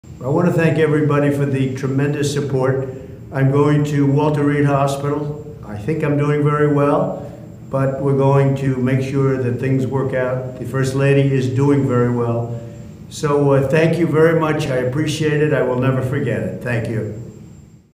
Trump releases video, says he's doing 'very well'
Speaking for the first time since his COVID diagnosis, President Donald Trump released a short video Friday evening saying that he thinks he's "doing very well." He says he's going to Walter Reed Medical Center "to make sure things work out." (Oct. 2)